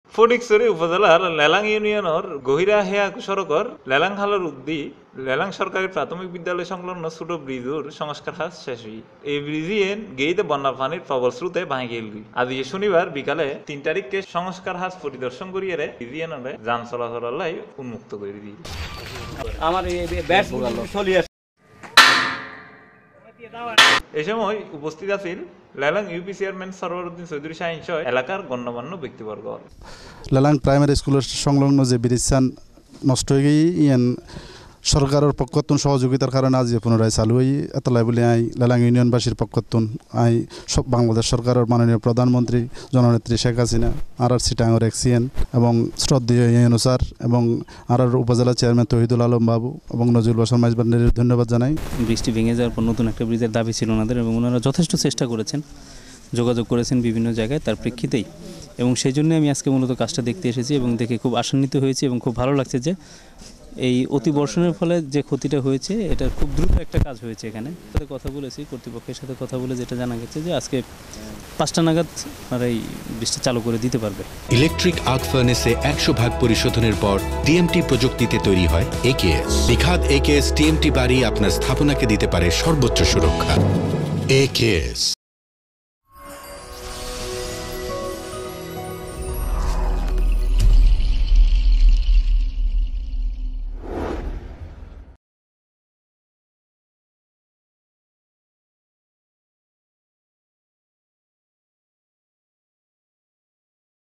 ফটিকছড়ির লেলাং এ ভেঙ্গে যাওয়া ছোট ব্রীজ সংস্কার শেষে যান চলাচল শুরু
ফটিকছড়ি উপজেলার লেলাং ইউপির গহিরা- হেয়াকোঁ সড়কের নয়াহাট বাজারের উত্তরে লেলাং খালের বাঁধ ভেঙ্গে বন্যার পানির প্রবল স্রোতে ভেঙ্গে যাওয়া লেলাং সরকারী প্রাথমিক বিদ্যালয় সংলগ্ন ছোট ব্রীজের সংস্কার কাজ শেষ হয়েছে। আজ শনিবার বিকাল ৩ টায় সংস্কার কাজ পরিদর্শন শেষে ফটিকছড়ি উপজেলা নির্বাহী অফিসার দীপক কুমার রায় আনুষ্টানিক ভাবে ব্রীজটিতে যান-চলাচলে জন্য উন্মুক্ত করে দেয়া হয়।এ সময় উপস্থিত ছিলেন লেলাং ইউপি চেয়ারম্যান সরোয়ার উদ্দীন চৌধুরী শাহিন, ভারপ্রাপ্ত চেয়ারম্যান সরোয়ার হোসেন, এ এস আই নাজমুল, ইউপি সদস্য মো: রফিক, ইফতেকার উদ্দীন মুরাদ, মো: সরোয়ার, মো: আইয়ুব, মো: আজম, রুমা আকতার, অনিতা রানী ও এলাকার গন্যমান্য ব্যক্তিবর্গরা।দীর্ঘদিন দুর্ভোগের পর যান- চলাচলে ব্রীজটি চালু হওয়ায় এলাকার হাজার হাজার মানুষের মনে স্বস্তি ফিরে এসেছে।